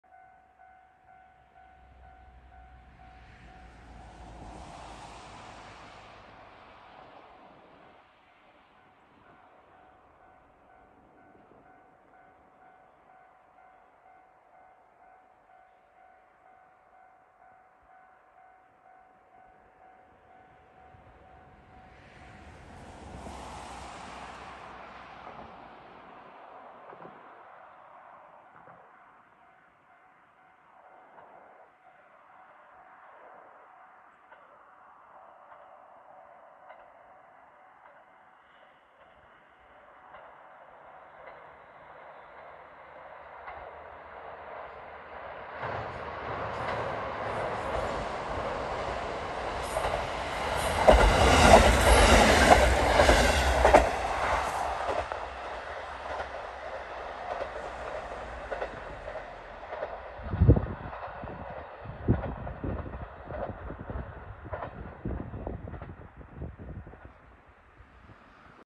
タラコ色重連‼️廃線間近の根室本線金山⇒下金山を走行するキハ40
ダイヤ改正より2両が所定となった富良野⇔東鹿越の列車に3月17日は、首都圏色同士の組み合わせで運転されました。